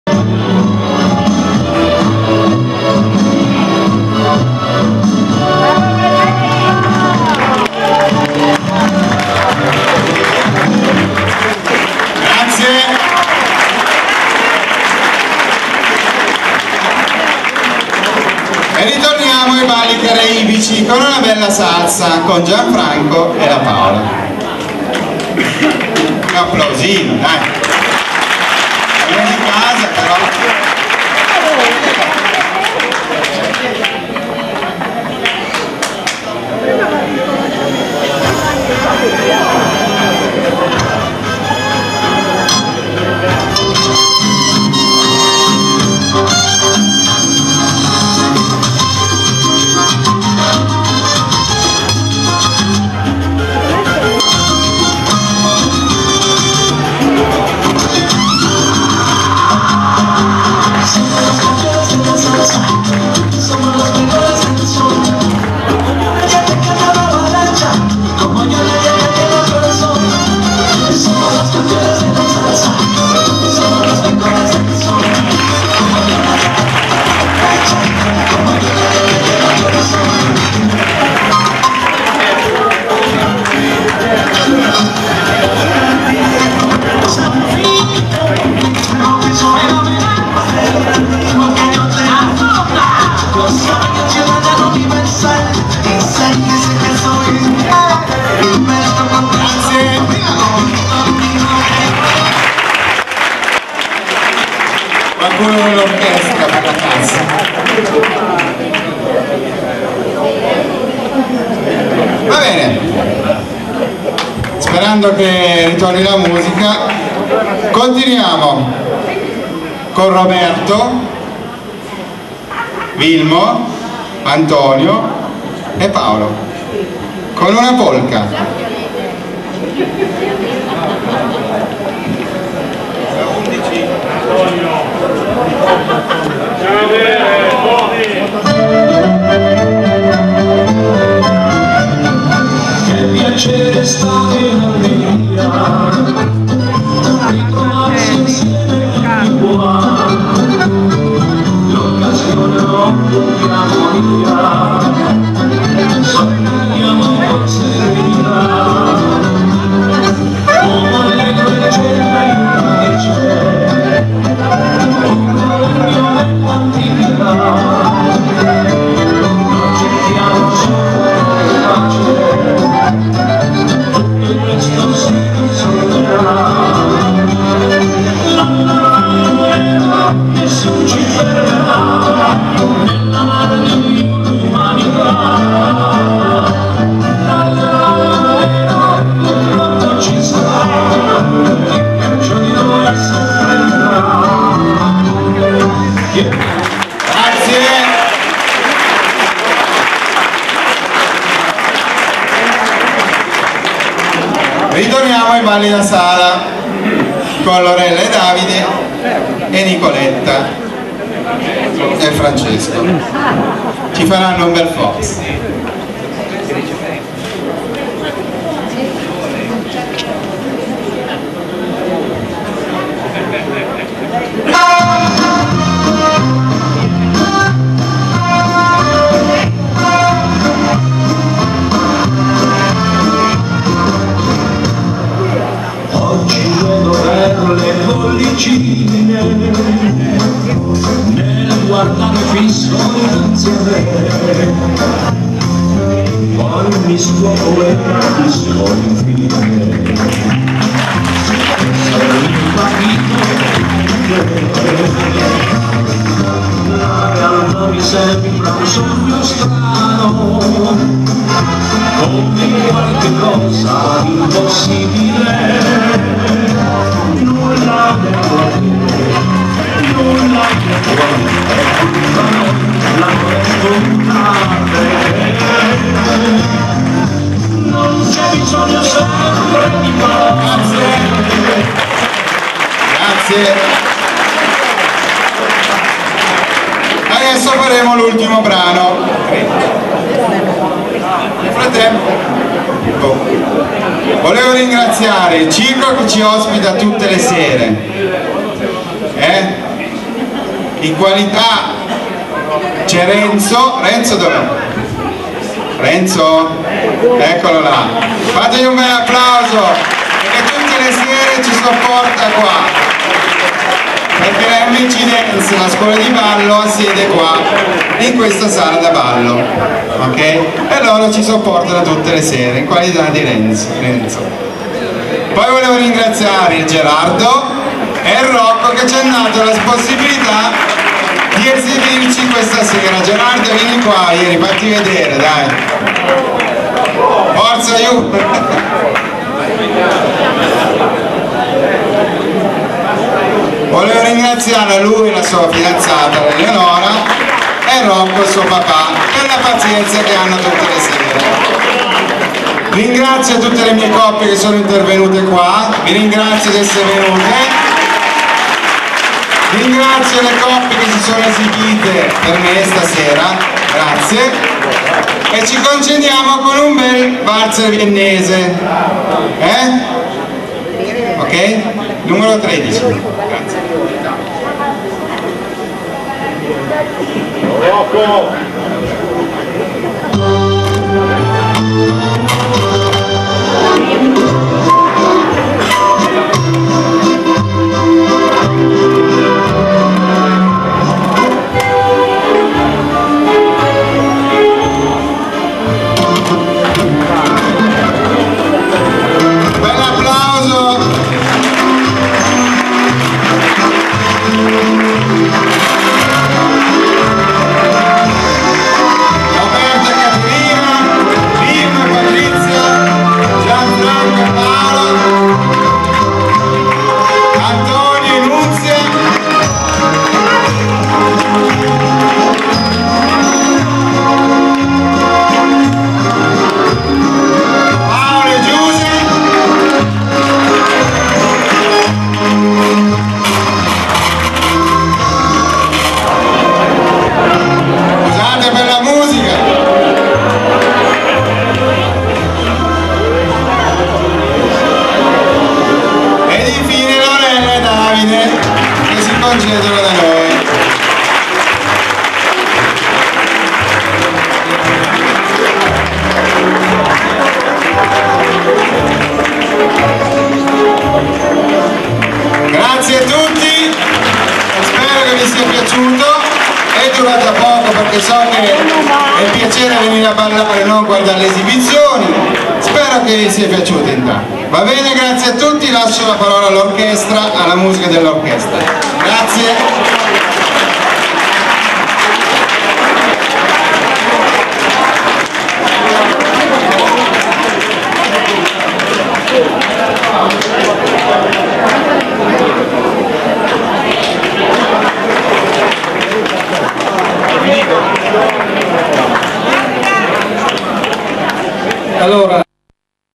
scuola di ballo Michele Asd M.C. Dance Casale Monferrato 2013
CHANELTommaso (Casale Monferrato) esibizione dei ballerini della scuola ASD M.C. DANCE del maestro Michele nella sala da ballo casaldanze